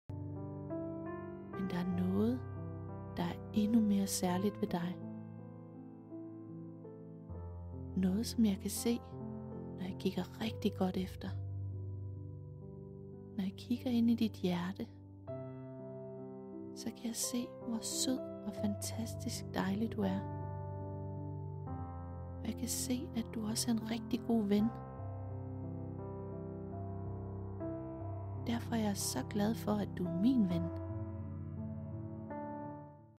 Lydprøve, Den Kloge Mariehøne
Den kloge mariehøne, er en rigtig selvværdsbooster til børn i alderen 4-11 år. Den indeholder også en guidet afslapning - eller meditation om du vil. download filen i webshoppen på